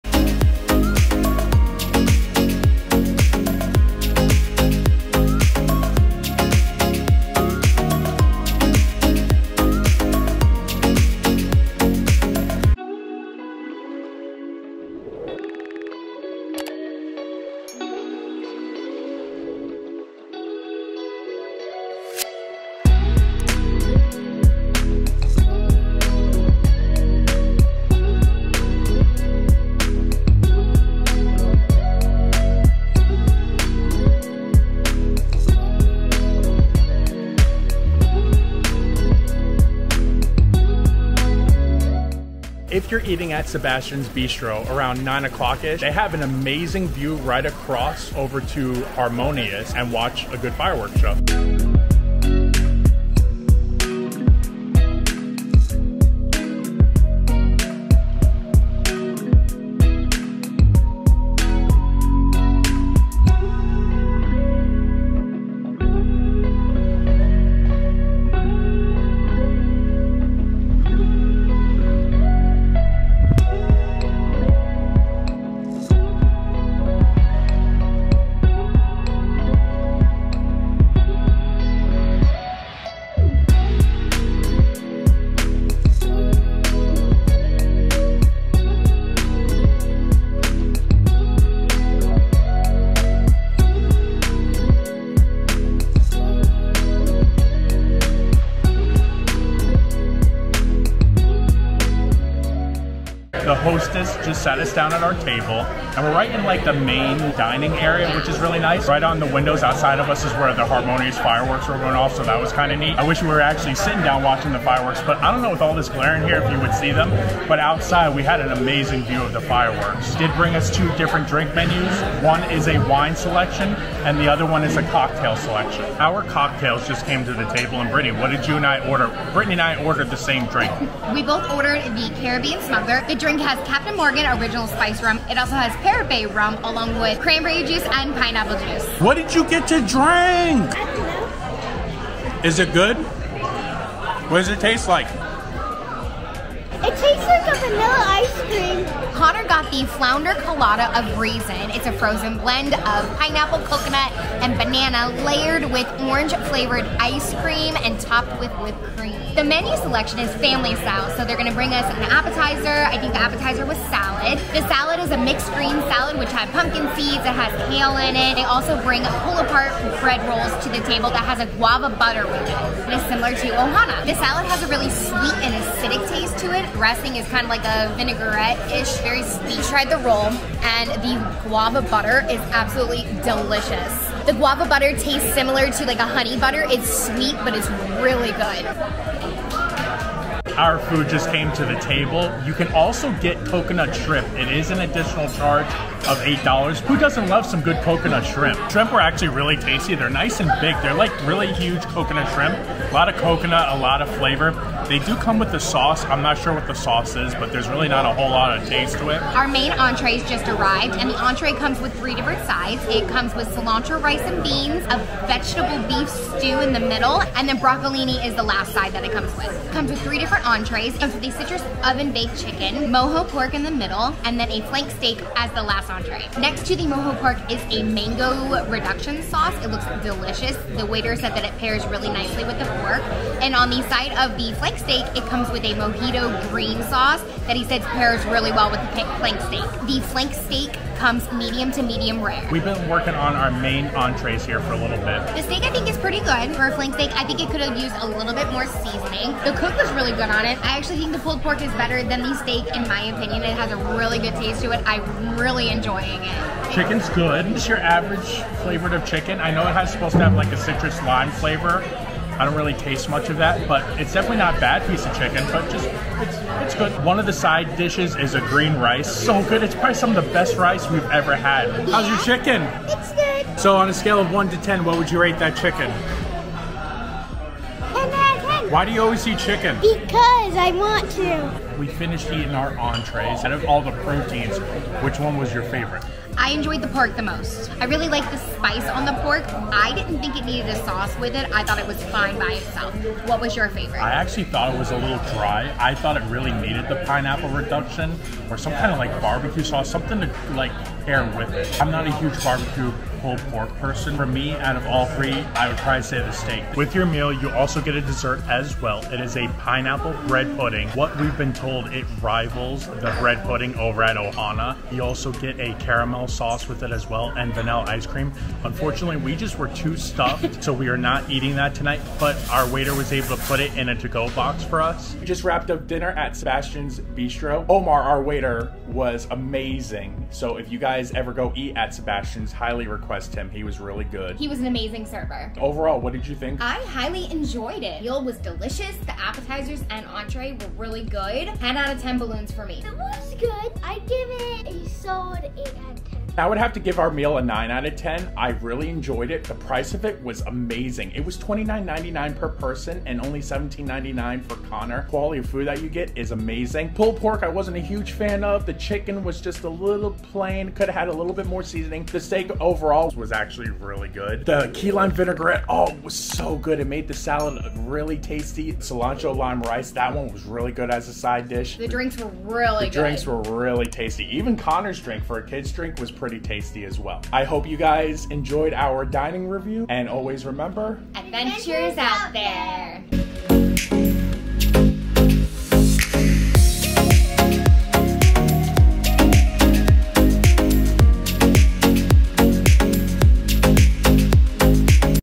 Disney World Dinner Review | Sebastians Bistro
Hey Friends,
We are back with another Disney World dining review. In this vlog we are visiting Disney Worlds Caribbean Beach resort and we are eating at Sebastians Bistro. This restaurant just recently reopened and we were very excited to try their family style menu.  Very similar to other family style restaurants on Disney property, the food is brought to your table.

Also,
& smash that 👍 button.

For more of our family content you can follow us on

Always Remember Adventure is Out There 🎈